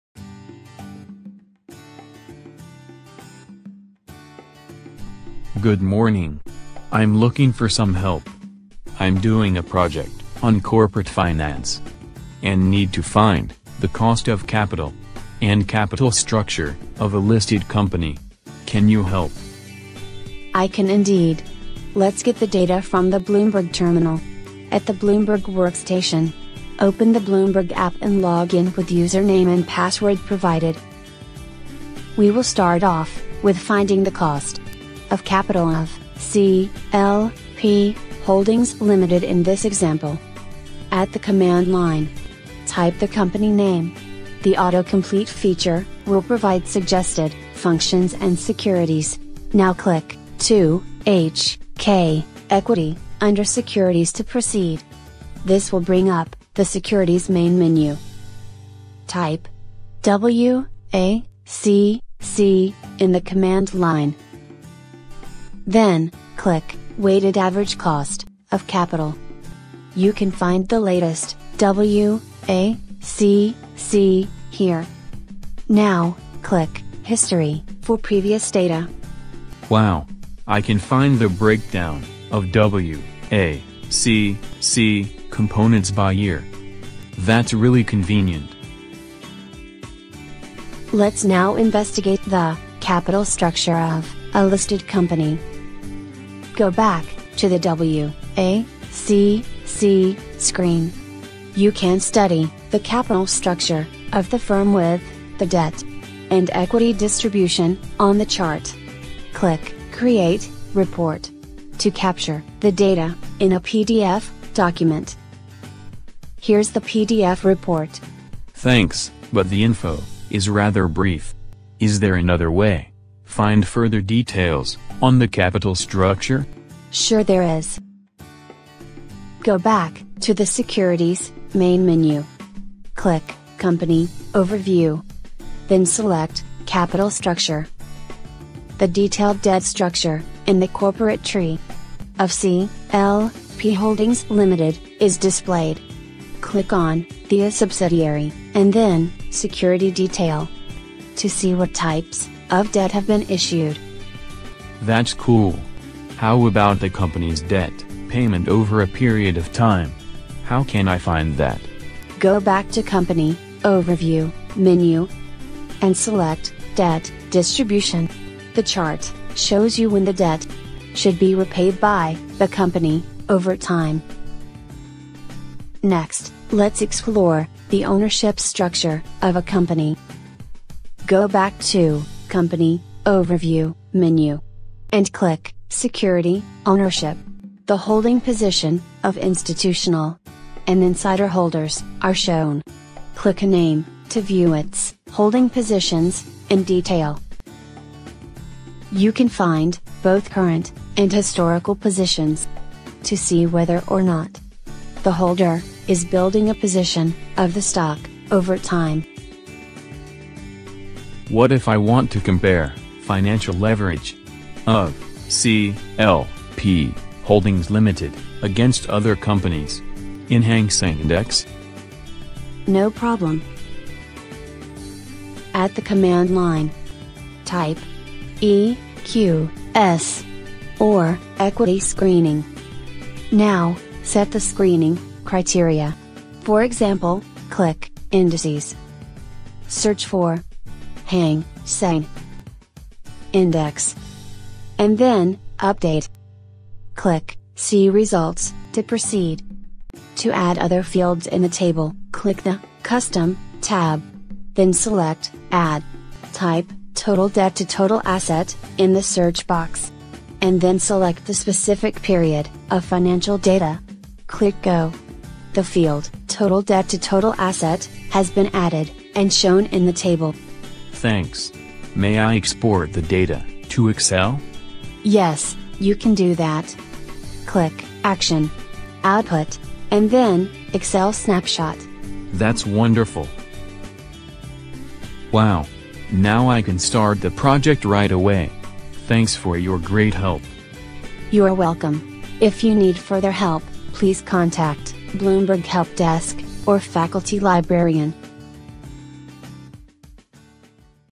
Finding Cost of Capital and Capital Structure
This Bloomberg video is prepared by Dr Anson Wong (AF), Dr Derek Yim (AF), and Mr William Ho (LIB) from the Hong Kong Polytechnic University, with funding support from UGC Teaching & Learning Project on enhancing information literacy in Hong Kong higher education through the development and implementation of shared interactive multimedia courseware (IL Project) from year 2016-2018.